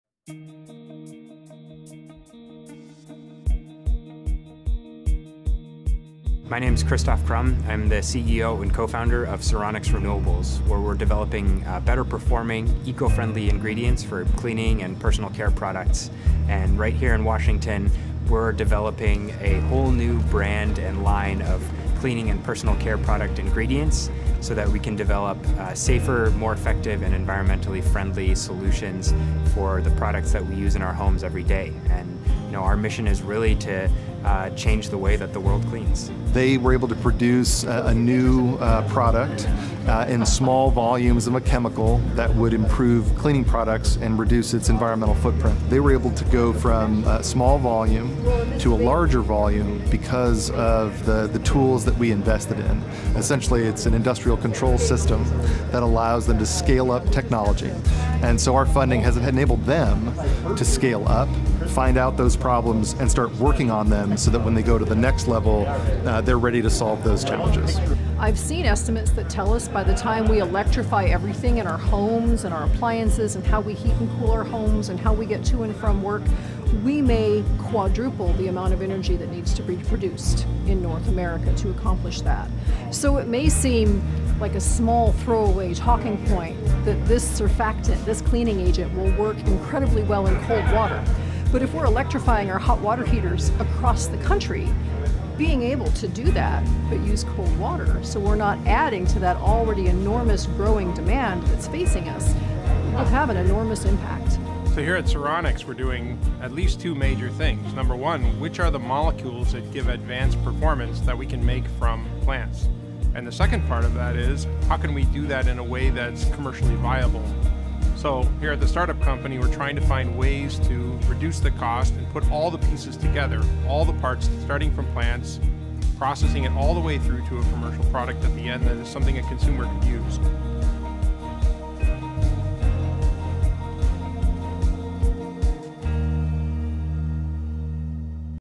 Sironix Renewables | A WA startup changing the way the world cleans
In-person ribbon cutting ceremony at the new Seattle headquarters, Sironix Renewables will celebrate expanded development facility, which further strengthen Sironix’s ability to scale production to meet growing customer demand.

In attendance:
Christoph Krumm - CEO & Co-founder of Sironix Renewables
Paul Dauenhauer - Co-founder of Sironix Renewables
Mel Clark from CleanTech Alliance
Michael Furze from the Department of Commerce | Head of Energy Division

• This project is an example of clean technology innovation in WA that has worldwide impact.
• The technology developed will strengthen communities by reducing energy consumption and emissions in the building sector – a core component of Washington’s State Energy Strategy.
• Affordable eco-products expand access to clean technologies for all Washington residents and help us achieve clean, affordable and equitable energy future.
• $234,450 from CEF3: to develop new, high-performance ingredients for cleaning products using natural oils and agricultural waste instead of petroleum.
• This new class of surfactants enables substantial expansion of the bio-renewable and bio-degradable cleaning products market. These surfactants are anticipated to outperform incumbent products and reduce the cost of formulating cleaning products.
• The product’s properties reduce energy consumption of consumer laundry by lowering water temperatures and reduce carbon emissions by cutting down on petroleum based ingredients.
• RD&D funding supported construction and development of a pilot plant to produce Sironix surfactants at a scale of 10 kg.
• Funding has enabled the company to transition from bench-scale to pilot scale in new facility and expand its partnership work with cleaning and personal brands toward launching better-performing, safer, and more environmentally friendly products.